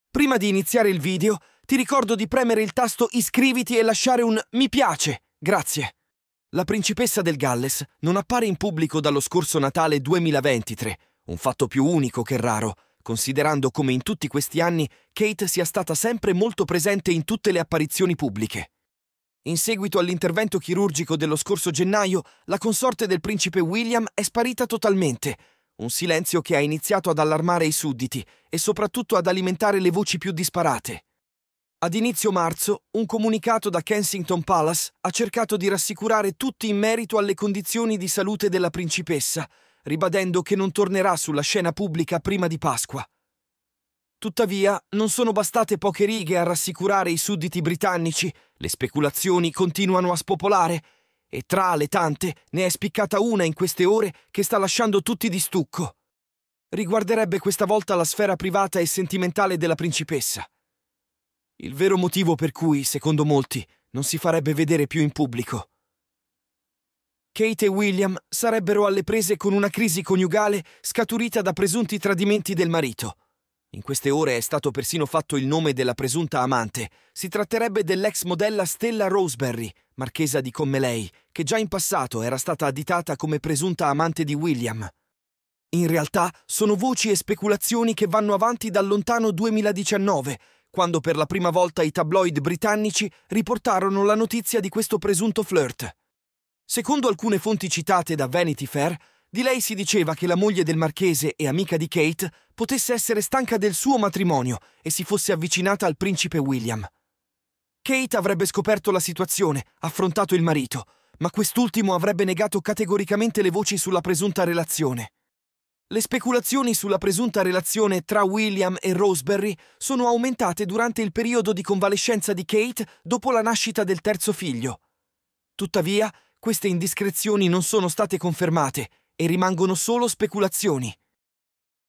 La notizia Shock su William e Kate - Sarà davvero lei l'amante del principe WIlliam??
Grazie. La principessa del Galles non appare in pubblico dallo scorso Natale 2023, un fatto più unico che raro, considerando come in tutti questi anni Kate sia stata sempre molto presente in tutte le apparizioni pubbliche. In seguito all'intervento chirurgico dello scorso gennaio, la consorte del principe William è sparita totalmente, un silenzio che ha iniziato ad allarmare i sudditi e soprattutto ad alimentare le voci più disparate.

Ad inizio marzo, un comunicato da Kensington Palace ha cercato di rassicurare tutti in merito alle condizioni di salute della principessa, ribadendo che non tornerà sulla scena pubblica prima di Pasqua. Tuttavia, non sono bastate poche righe a rassicurare i sudditi britannici, le speculazioni continuano a spopolare e tra le tante, ne è spiccata una in queste ore che sta lasciando tutti di stucco.

Riguarderebbe questa volta la sfera privata e sentimentale della principessa. Il vero motivo per cui, secondo molti, non si farebbe vedere più in pubblico. Kate e William sarebbero alle prese con una crisi coniugale scaturita da presunti tradimenti del marito. In queste ore è stato persino fatto il nome della presunta amante, si tratterebbe dell'ex modella Stella Roseberry, marchesa di Conmelei, che già in passato era stata additata come presunta amante di William.

In realtà, sono voci e speculazioni che vanno avanti dal lontano 2019, quando per la prima volta i tabloid britannici riportarono la notizia di questo presunto flirt. Secondo alcune fonti citate da Vanity Fair, di lei si diceva che la moglie del marchese e amica di Kate potesse essere stanca del suo matrimonio e si fosse avvicinata al principe William. Kate avrebbe scoperto la situazione, affrontato il marito, ma quest'ultimo avrebbe negato categoricamente le voci sulla presunta relazione.

Le speculazioni sulla presunta relazione tra William e Roseberry sono aumentate durante il periodo di convalescenza di Kate dopo la nascita del terzo figlio. Tuttavia, queste indiscrezioni non sono state confermate e rimangono solo speculazioni.